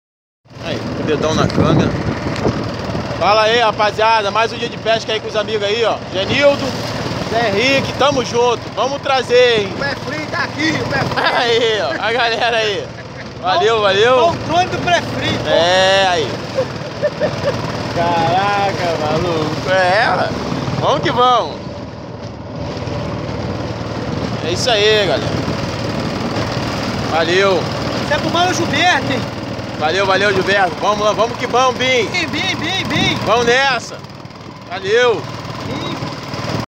pescaria partiu ..
manois lauro ,genildo e henrique ,,partindo para mais uma pescaria noturna ,na baia de uanabara.